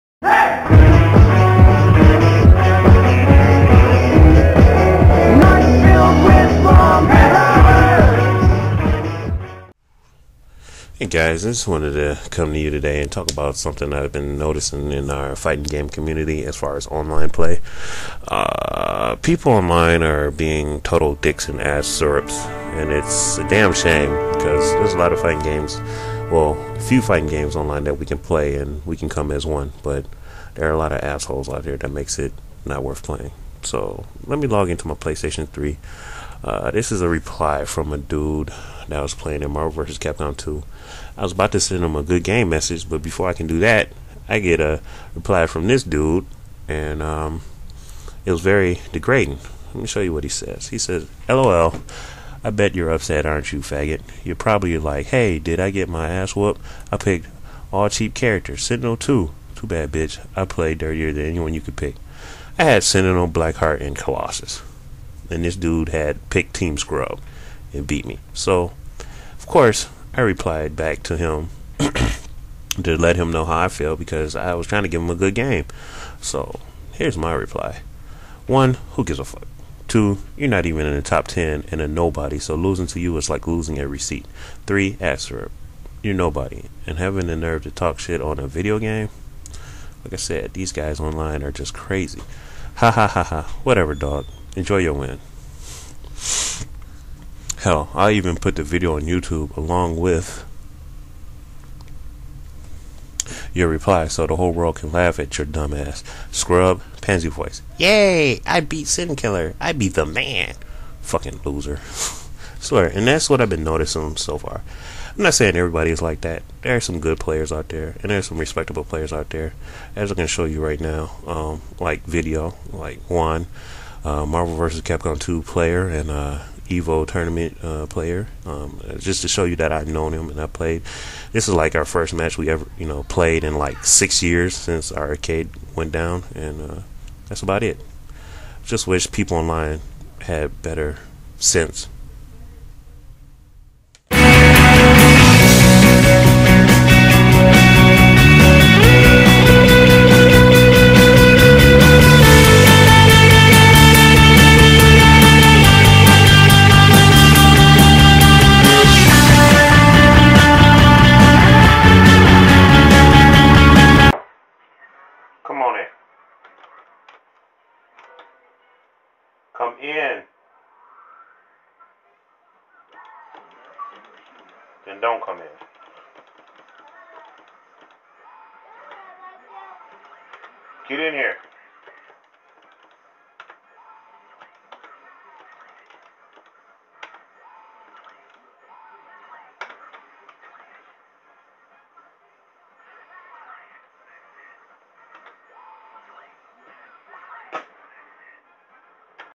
DoaMG: There's a distrubance in the force
Just something I found disturbing in the fighting game community.  Rage quitting, Rage emails, etc.